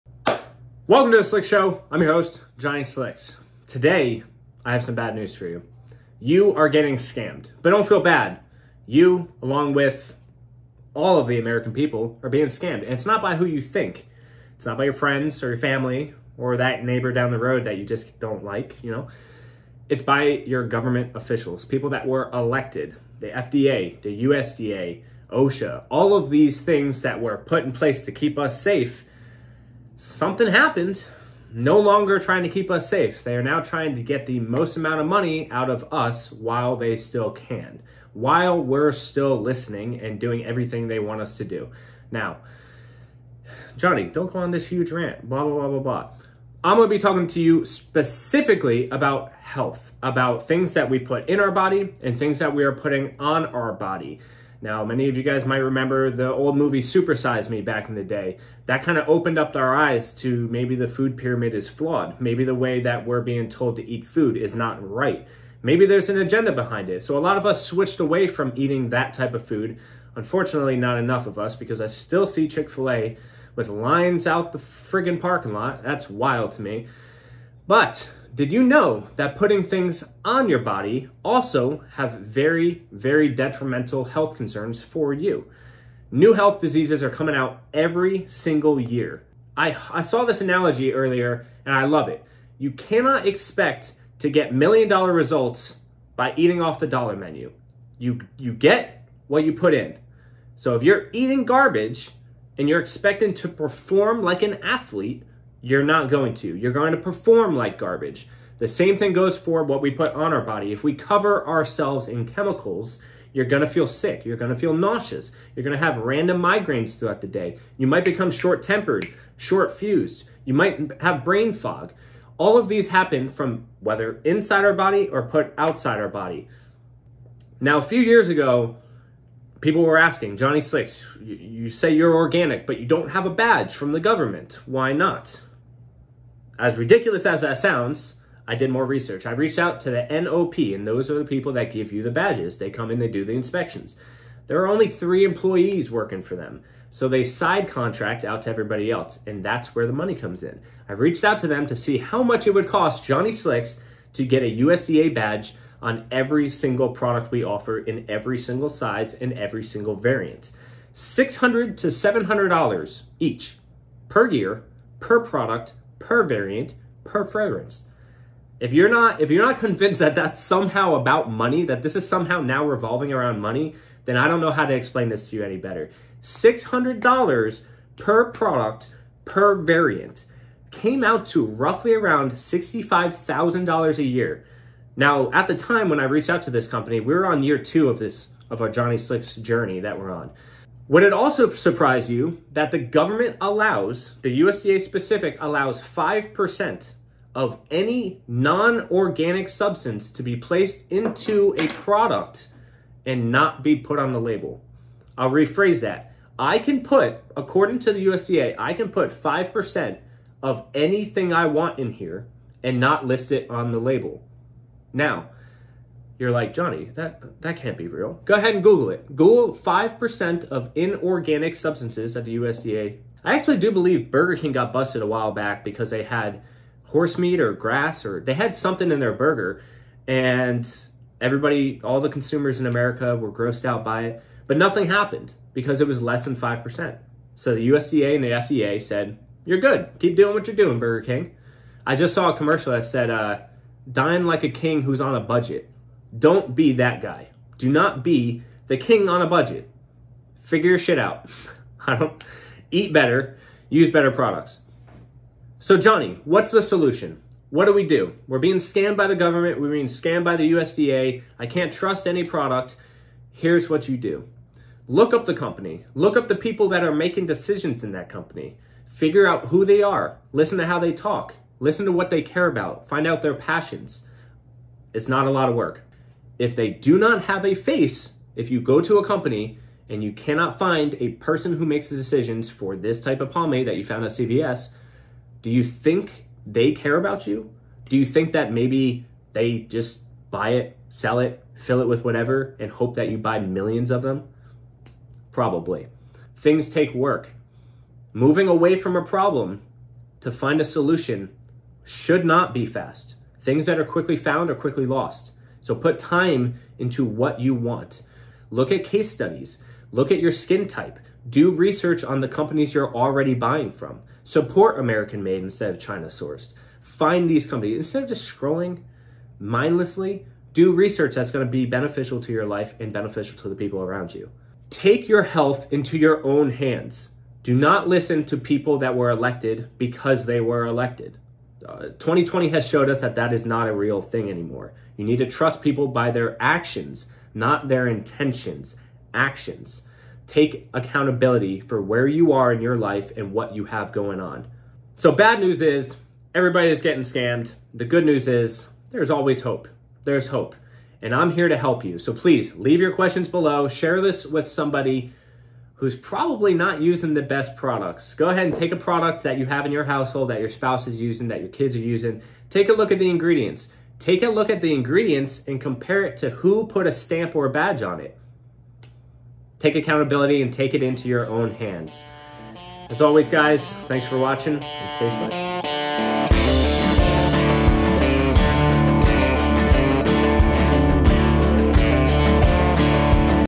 You're Being Scammed! | The Slick Show Episode 001
Welcome To The Slick Show!
Johnny Slicks will answer common questions, discussing trending topics regarding being organic, American Made, and health.